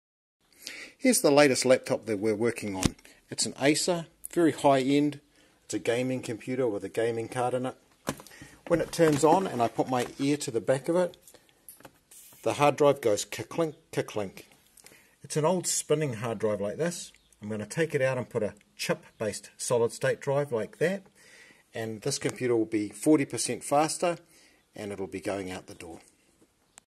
Laptop makes clicking noises and won't startup.
Laptop makes clicking noises and won't startup. Here is today's repair. If we can help you with your technology get in contact with us.
Facebook.com/rexshortcomputerservices